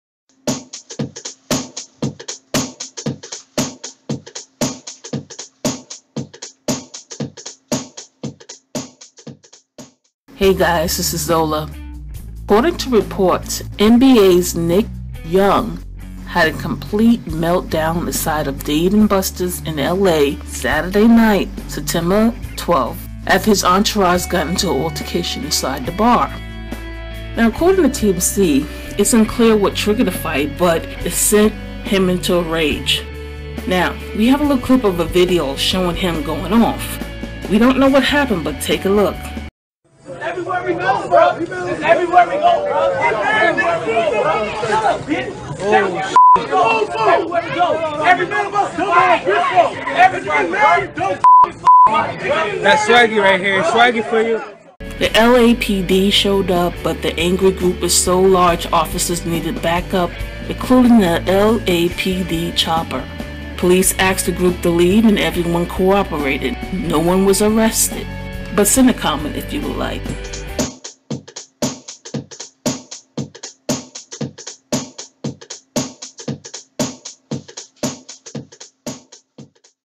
NICK YOUNG Altercation At Dave & Buster's in L.A. [VIDEO & DISCUSSION]
According to TMZ, Nick Young had a complete MELTDOWN outside of Dave & Buster's in L.A. Saturday night (September 12) ... after his entourage got into an altercation inside the bar ... TMZ Sports has learned.
It's unclear what triggered the fight, but it sent Swaggy into a RAGE. The video shows Young ripping into his buddy, accusing him of instigating the drama ... then chucking a water bottle in his direction.
LAPD showed up, but the angry group was so large officers needed backup, including an LAPD chopper.
Cops asked the group to leave and everyone cooperated.   No was was arrested.

Yours truly,

CelebrityTV

Nick Young, altercation, brawl, Dave & Busters L.A., entourage, fight, NBA, Nick Young altercation, CelebrityTV